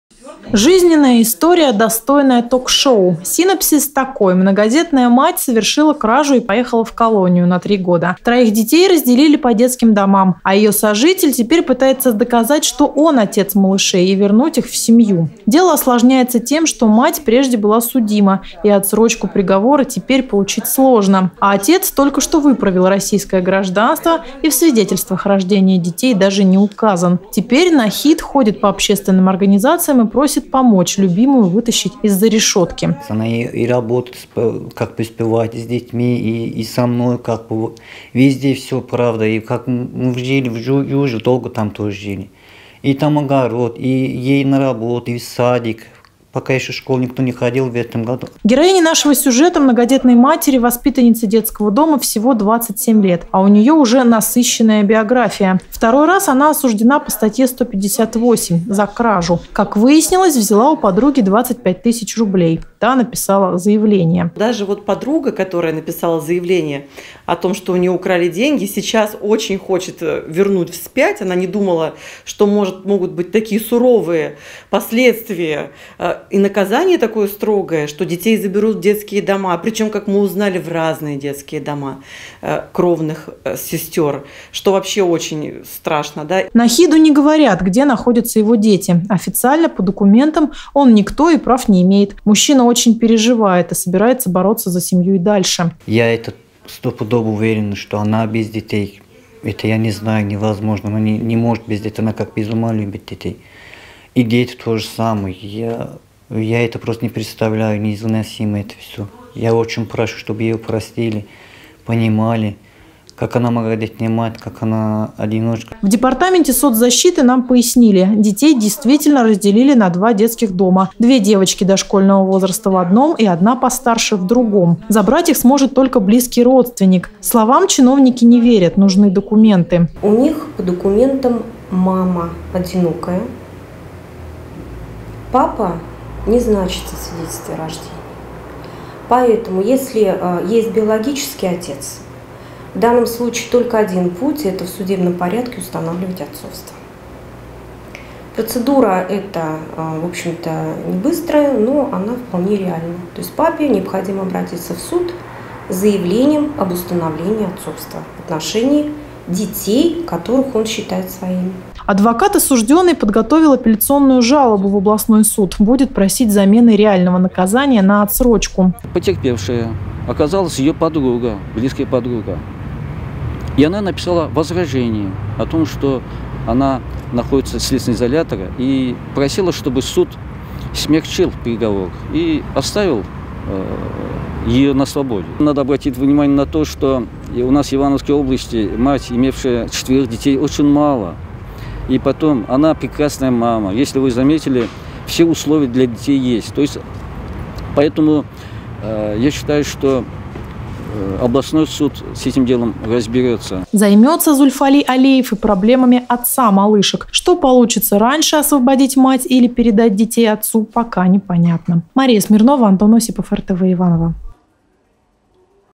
Жизненная история, достойная ток-шоу
Мать сидит, отец пытается оформить опеку над детьми, которых считает своими, адвокаты добиваются смягчения наказания. Звучит как сценарий для сериала, а на деле — жизненная история.